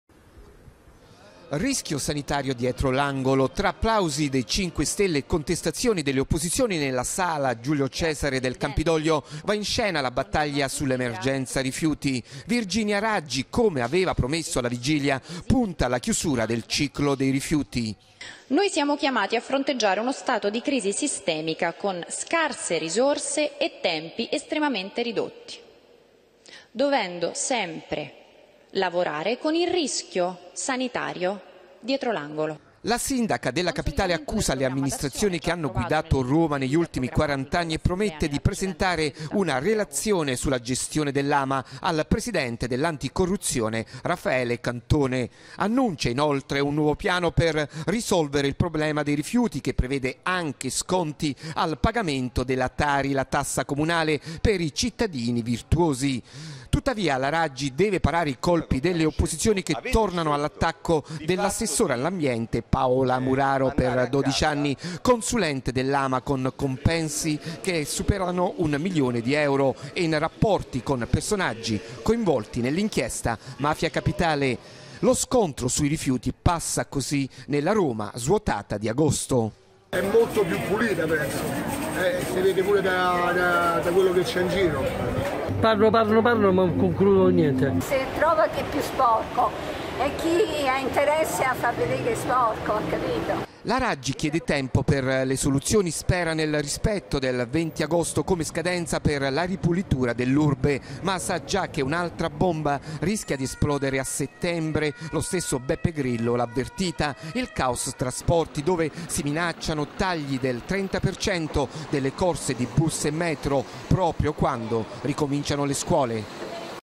Rifiuti a Roma: la sindaca Raggi:"Rischio sanitario dietro l'angolo"
Rifiuti a Roma: la sindaca Raggi:"Rischio sanitario dietro l'angolo". Servizio di Augusto Cantelmi